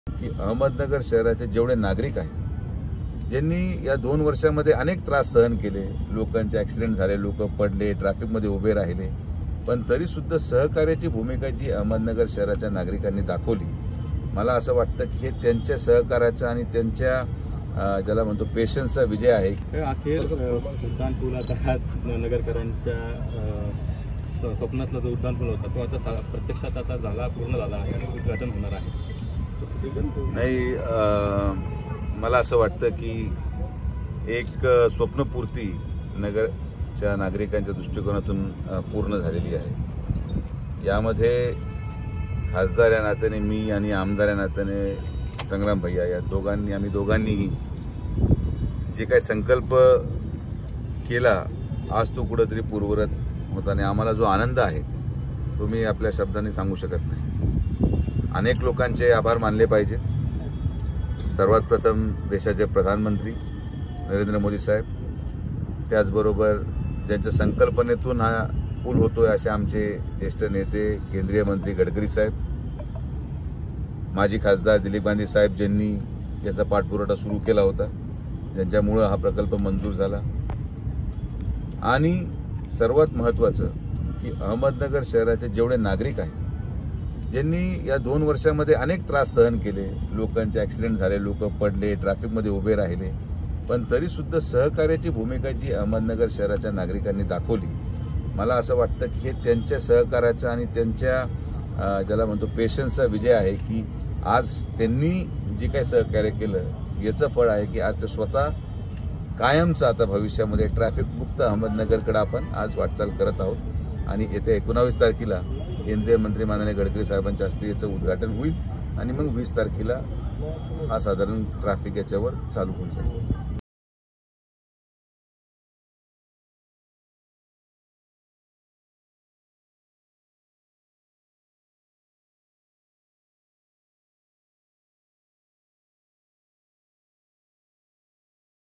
Ahmednagar News : सुजय विखे-संग्राम जगताप एकत्र पोहचले उड्डाणपुलावर | LetsUpp Marathi
अहमदनगर शहरातील उड्डाणपुलाच्या कामाची पाहणी करण्यासाठी भाजपचे खासदार डॉ. सुजय विखे पाटील आणि राष्ट्रवादीचे आमदार संग्राम जगताप हे एकत्र दिसून आले. विशेष बाब म्हणजे एकाच गाडीतून त्यांनी या उड्डाणपुलाची पाहणी केली.